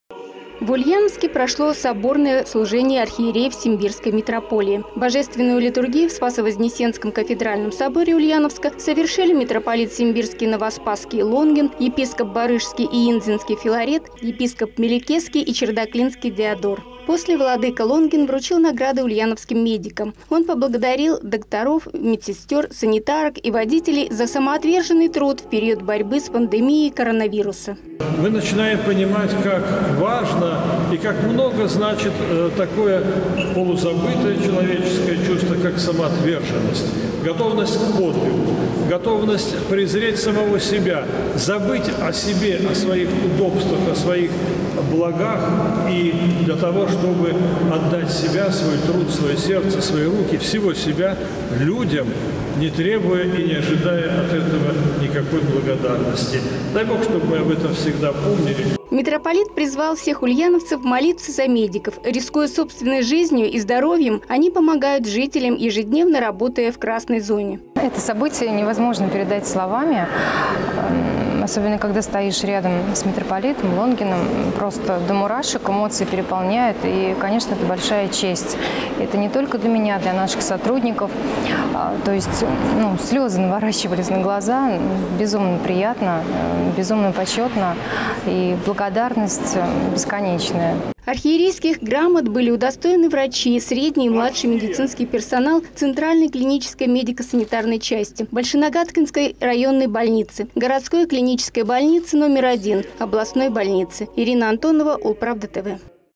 Митрополит Лонгин вручил архиерейские награды медикам Ульяновской области
В Ульяновске прошло соборное служение архиереев Симбирской митрополии.